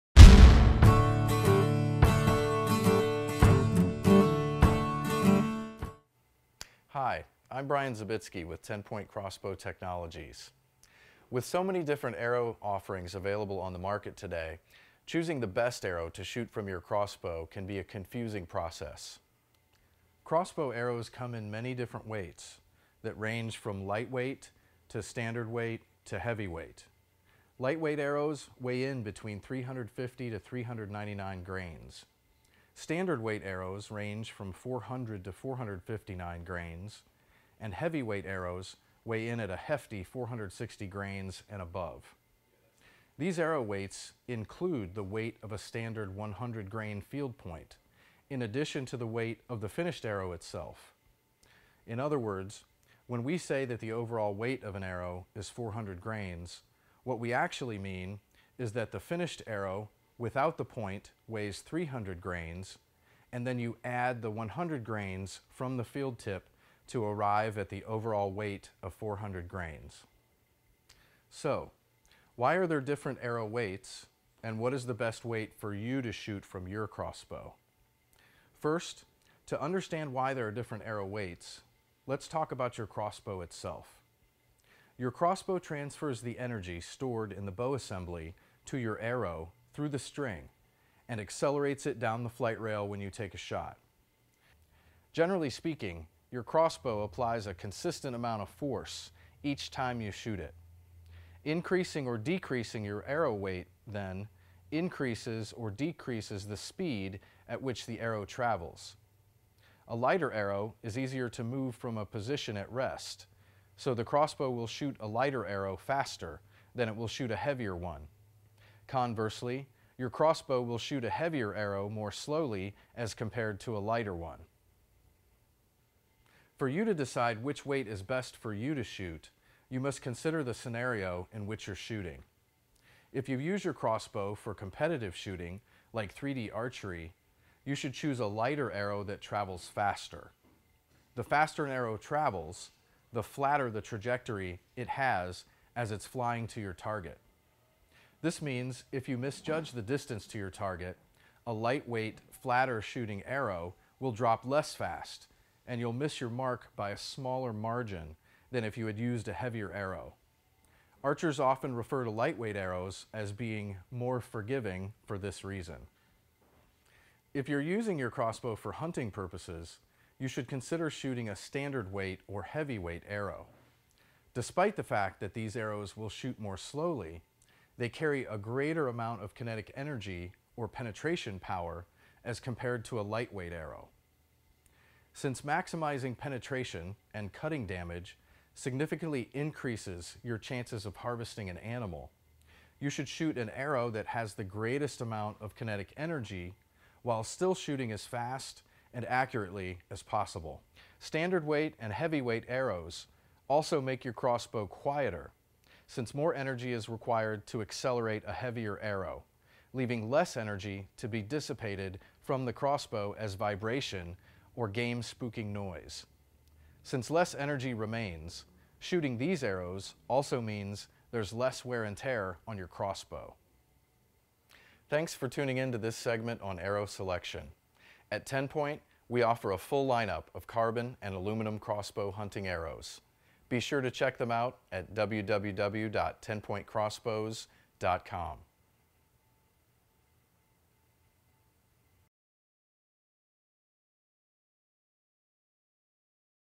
Crossbow Tech Tip: How To Choose Heavy Vs. Light Crossbow Arrows | TenPoint Crossbows
Should you shoot a light crossbow arrow to achieve the fastest arrow speed from your crossbow or should you shoot a heavy crossbow arrow to maximize kinetic energy?

In this tech tip, we discuss light, standard, and heavy weight arrows and the differences between them to help you make the best arrow weight decision for your crossbow hunting setup.